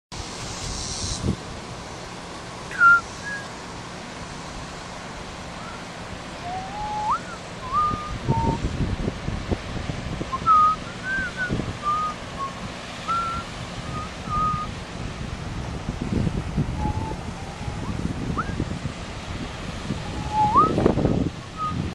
Branson From Butterfly Palace
View of Branson from the Butterfly Palace.  Recorded on August 6, 2010 using a Flip Video camcorder.